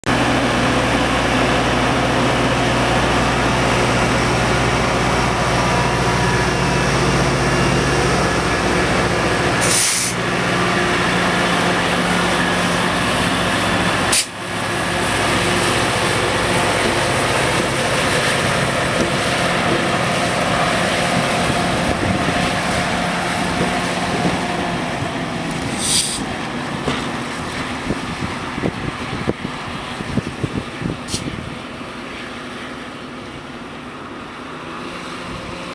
Departure of The Skeena train from Jasper to Prince George
Departure of The Skeena train from Jasper to Prince George on 09th January 2009